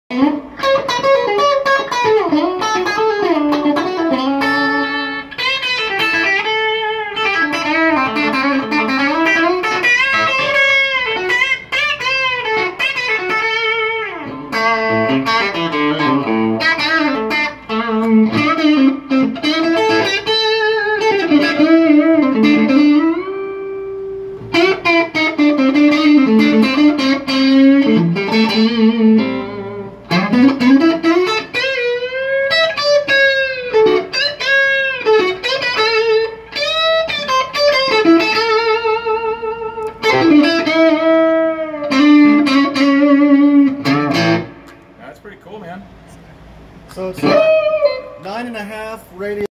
All Mesquite Stratocaster
Jacob at Charley's Guitar Shop in Dallas is taking it out for a spin.